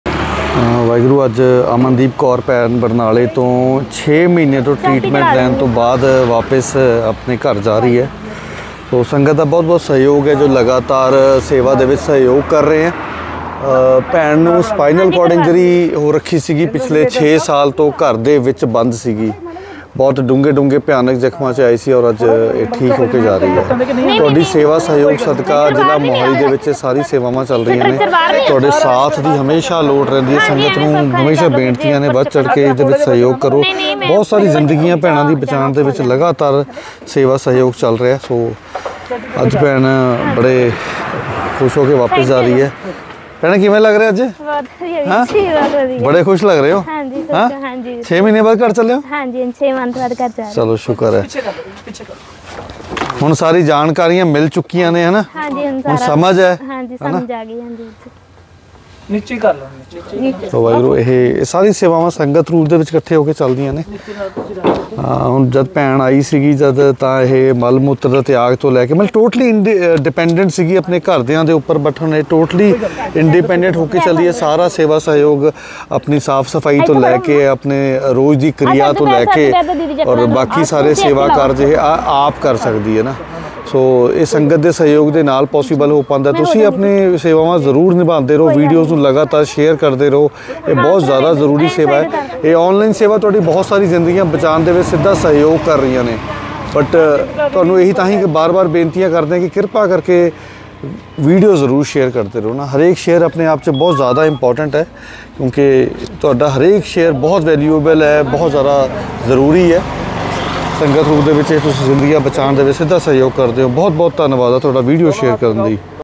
Spinal Cord ਤੋਂ ਪੀੜਤ ਇੱਕ ਹੋਰ ਭੈਣ ਦਾ ਸਫਲ ਇਲਾਜ਼ ਕਰ ਘਰ ਭੇਜਿਆ | Serve Humanity Serve God
Spinal Cord ਤੋਂ ਪੀੜਤ ਇੱਕ ਹੋਰ ਭੈਣ ਦਾ ਸਫਲ ਇਲਾਜ਼ ਕਰ ਘਰ ਭੇਜਿਆ